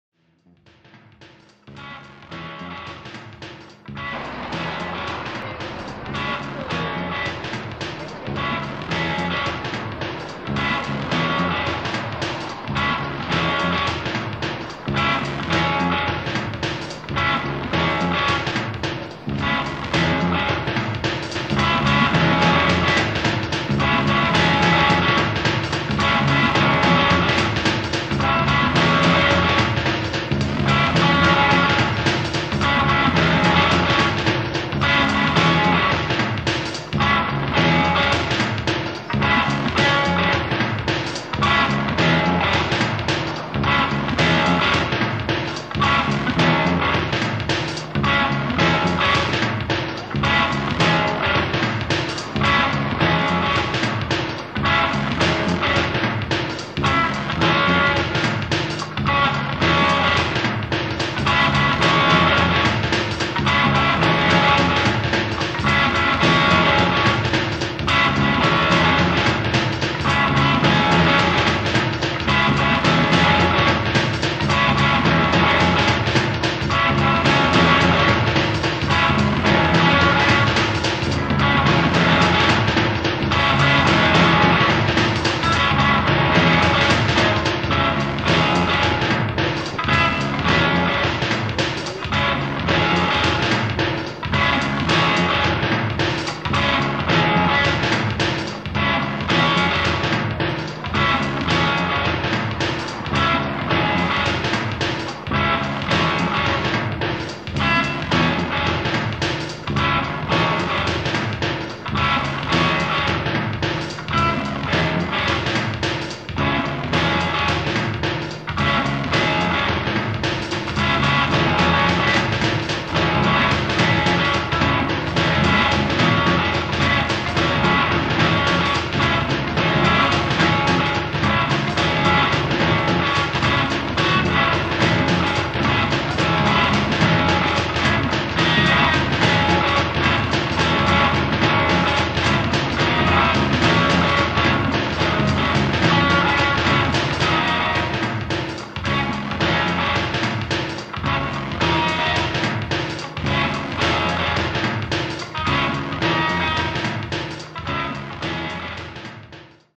A street in Denmark
Pedestrians, cars and bicycles at a heavy pace in the danish capital, accompanied by an old jam from the nineties by the band Ingenjör Lindell (Engineer Lindell) consisting of Ola Carlberg, Stefan Nilsson and myself.